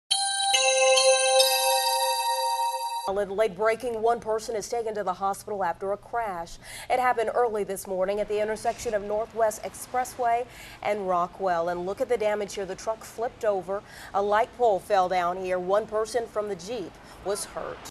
Jeep & Truck Crash, Truck Flips
A jeep and truck crash at Northwest Expressway and Rockwell and the truck flipped. One person was taken to the hospital.